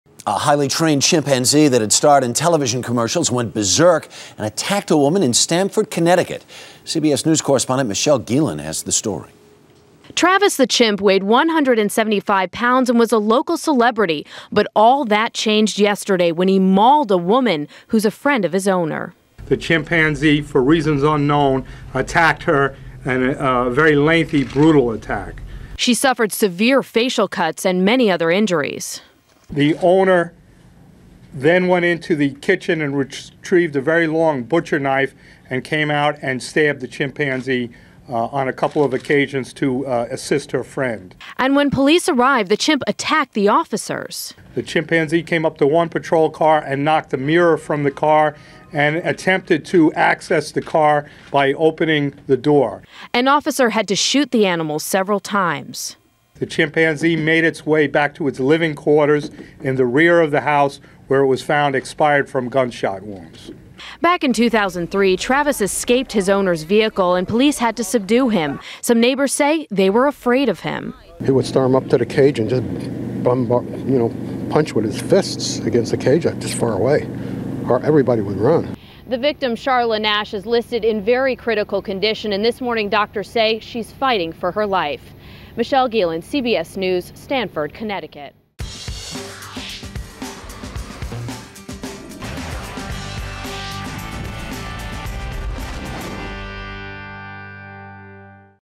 Chimp Severely Mauls Woman
Police had to shoot a chimpanzee three times after it attacked a woman, leaving her in critical condition and going after authorities during a violent rampage, Michelle Gielan reports.